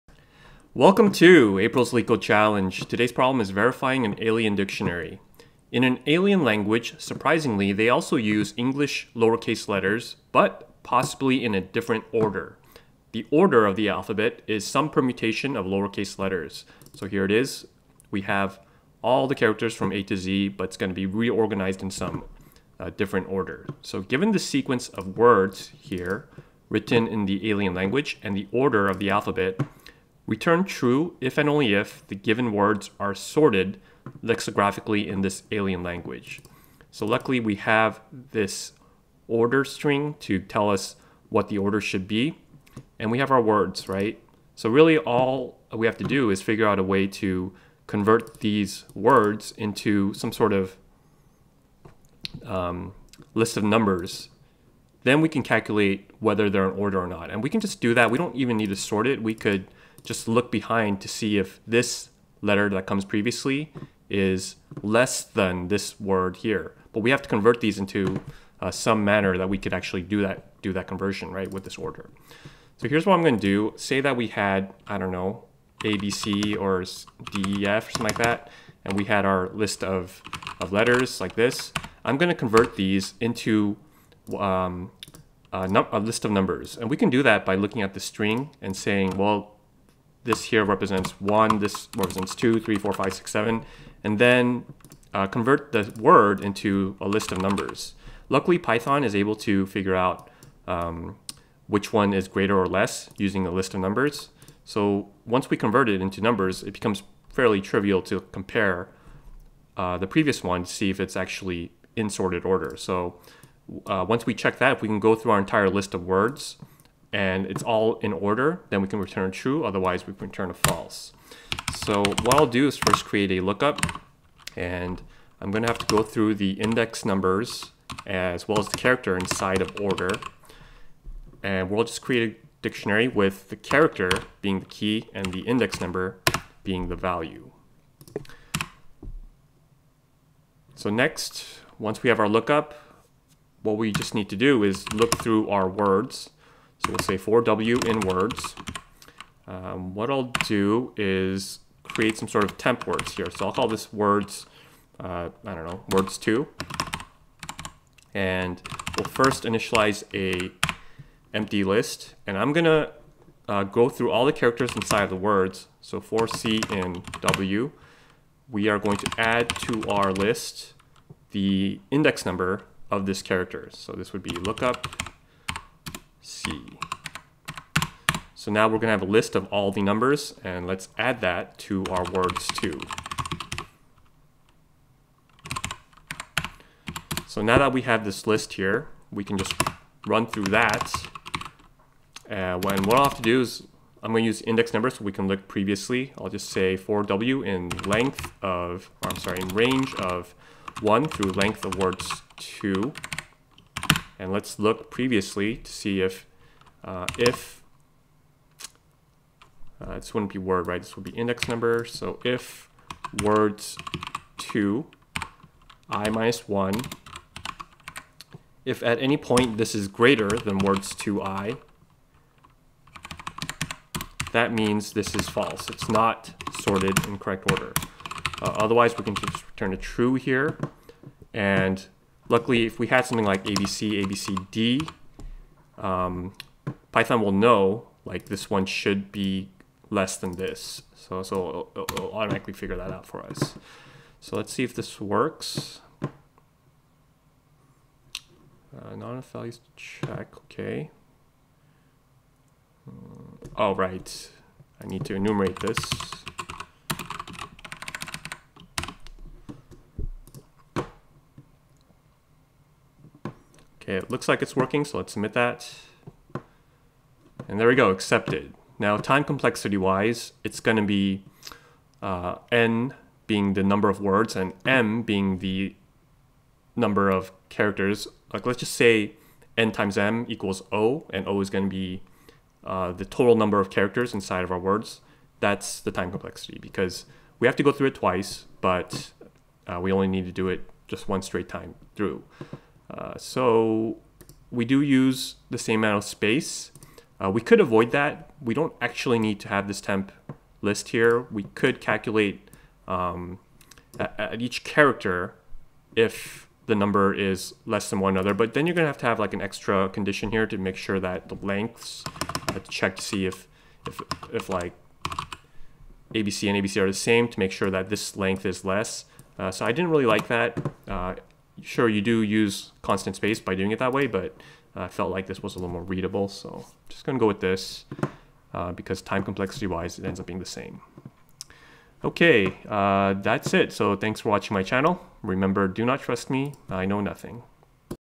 Leetcode - Verifying an Alien Dictionary (Python)
April 2021 Leetcode Challenge
Leetcode - Verifying an Alien Dictionary #953
Difficulty: Easy